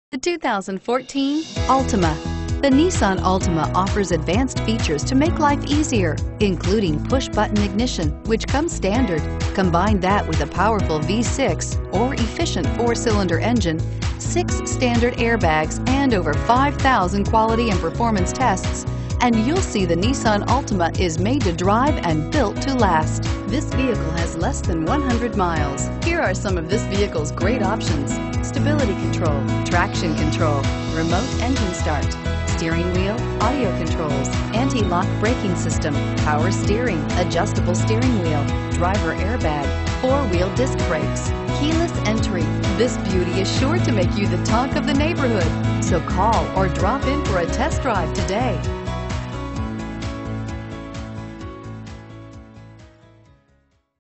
2014 Nissan Altima Fort Collins CO 240884
The 2014 Nissan Altima 2.5 SV features the 2.5 liter 4 cylinder that produces 182 HP.The Altima 2.5 SV is equipped with 17-inch alloy wheels, dual-zone automatic climate control, a leather-wrapped steering wheel, remote ignition, 5.0-inch touchscreen display, a rearview camera, a USB port, satellite radio and NissanConnect Apps.Safety features include antilock brakes, stability and traction control, front side airbags and side curtain airbags.